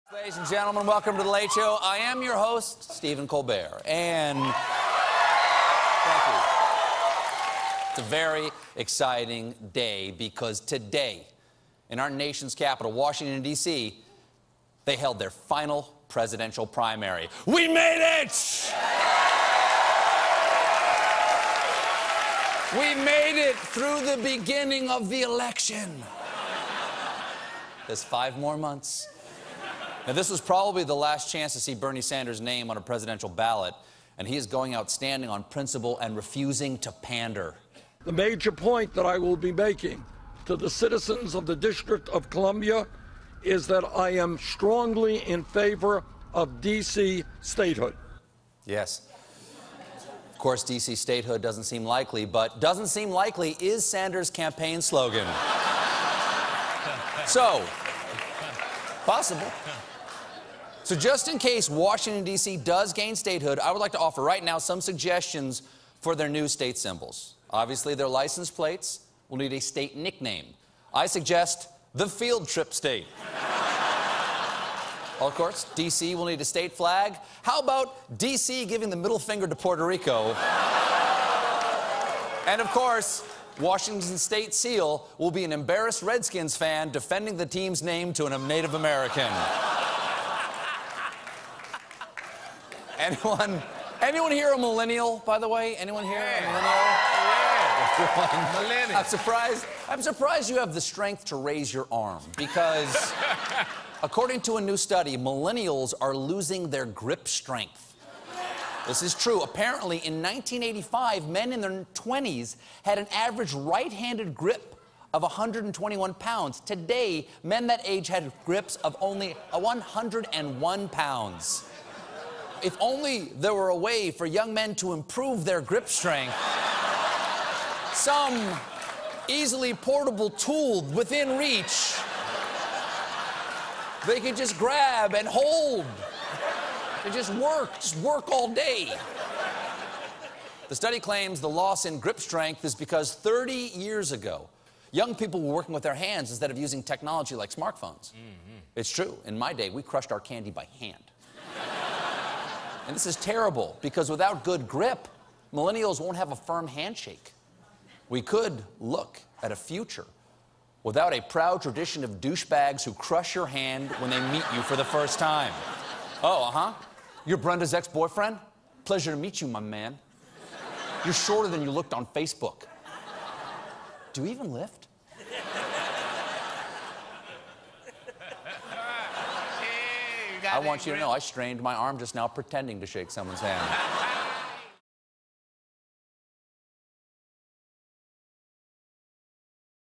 Washington, DC Could Become A State, and Millennials Are Losing Their Grips
Bernie Sanders comes out in favor of statehood for our nation's capitol, and Stephen examines a new medical study concerning millennials.